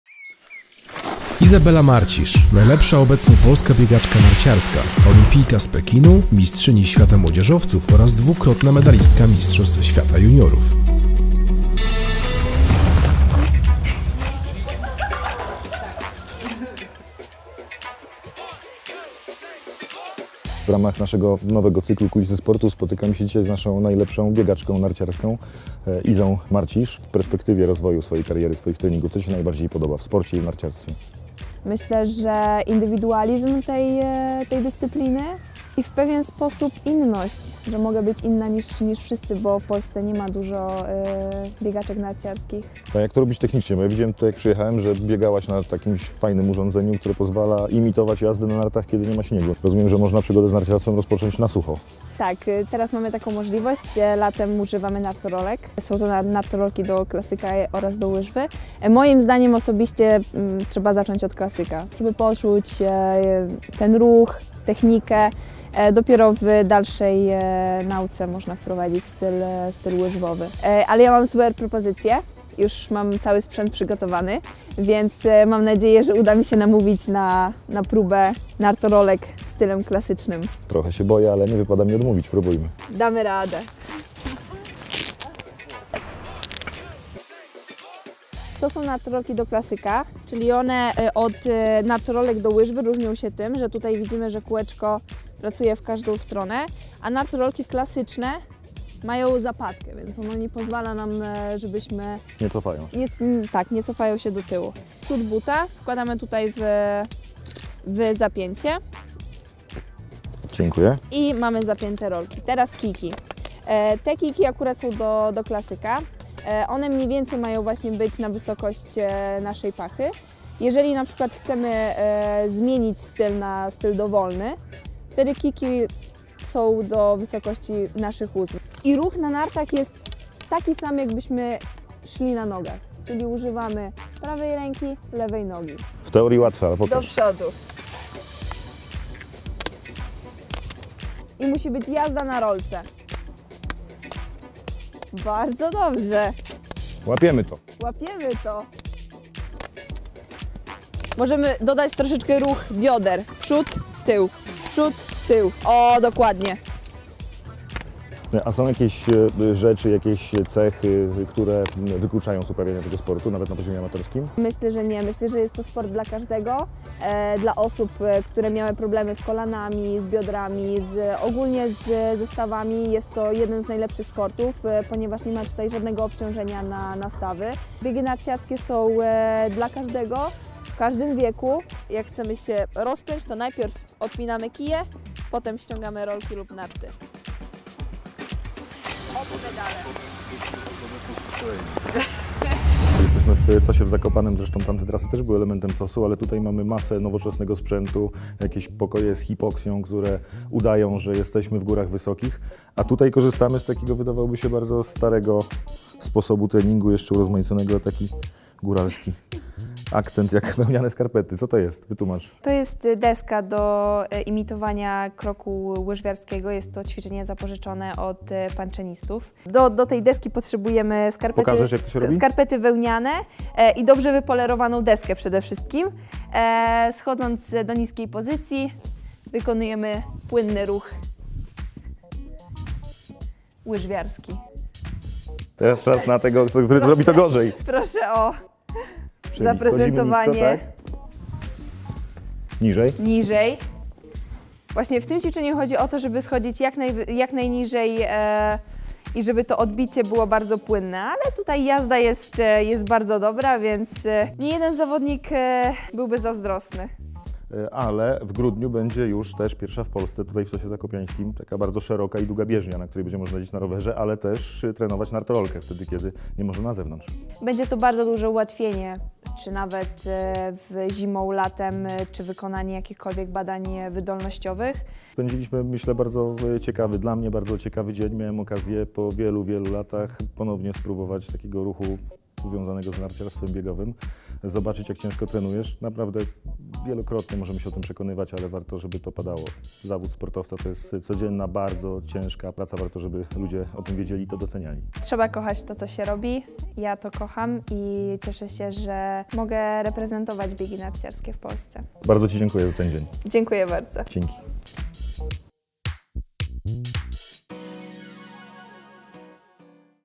Kulisy Sportu - Kamil Bortniczuk i Iza Marcisz pokazują, jak zacząć przygodę z biegami narciarskimi
Ferie zimowe to świetny czas, aby postawić na aktywność fizyczną! Minister sportu i turystyki Kamil Bortniczuk podczas wyjątkowego treningu z Izabelą Marcisz, młodzieżową wicemistrzynią świata, pokazuje jak rozpocząć przygodę z biegami narciarskimi. Zapraszamy za Kulisy Sportu!